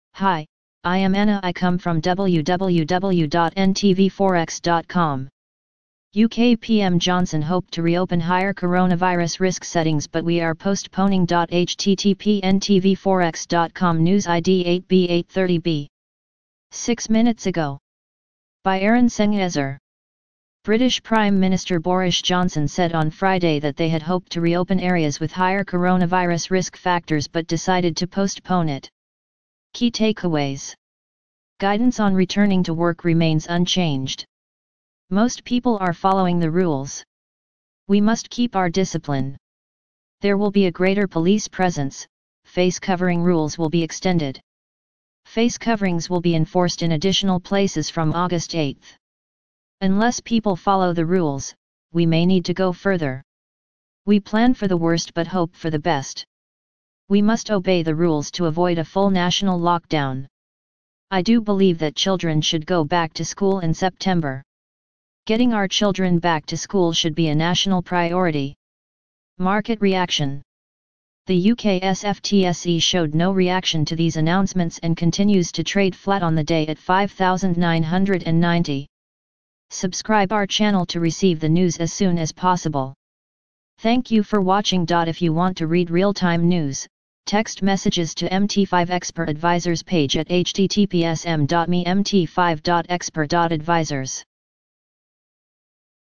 UK PM Johnson Hoped to reopen higher coronavirus risk settings but we are postponing
. 6 minutes ago . By Eren Sengezer . British Prime Minister Borish Johnson said on Friday that they had hoped to reopen areas with higher coronavirus risk factors but decided to postpone it. Key takeaways . Guidance on returning to work remains unchanged. Most people are following the rules. We must keep our discipline. There will be a greater police presence, face covering rules will be extended. Face coverings will be enforced in additional places from August 8. Unless people follow the rules, we may need to go further. We plan for the worst but hope for the best. We must obey the rules to avoid a full national lockdown. I do believe that children ...